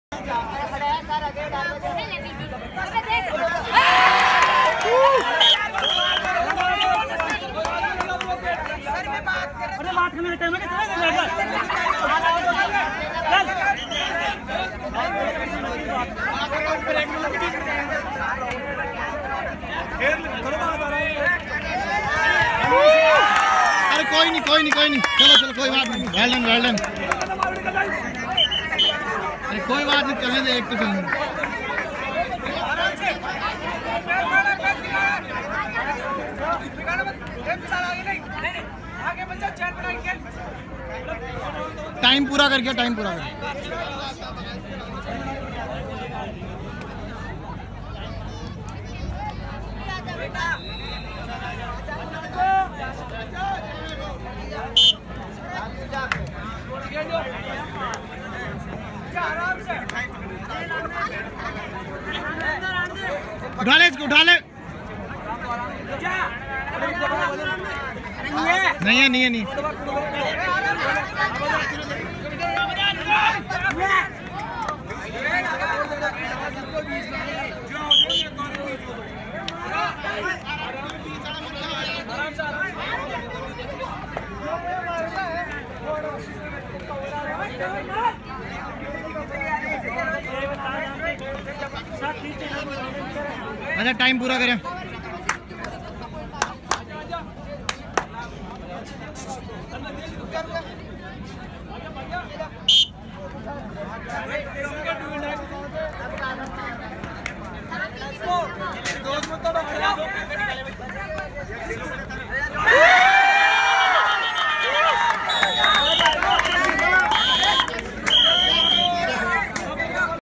KABADDI V/S KESHAV. ROCKS WITH 36 POINTS IN 63rd STATE LEVEL GOVT TOURNAMENT
Keshav public sr sec school KABADDI PLAYERS.

ROCKS WITH 36 POINTS IN 63rd STATE LEVEL GOVT TOURNAMENT